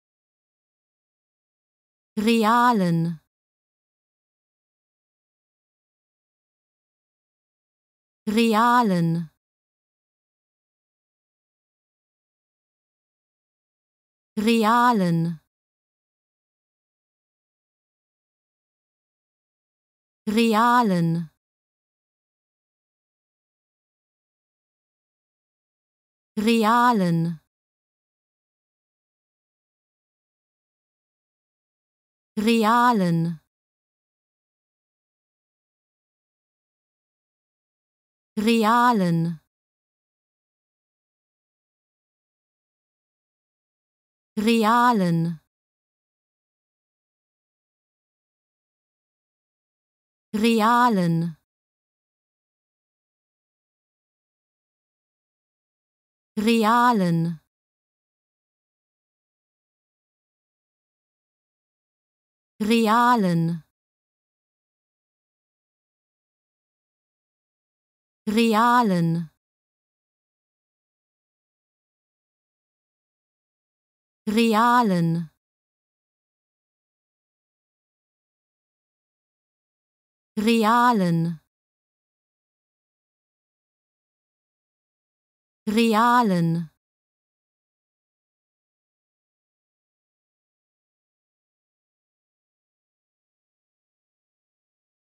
How to Pronounce ''realen'' (real) Correctly in German
Learn how to say ''realen'' in German with this short pronunciation tutorial.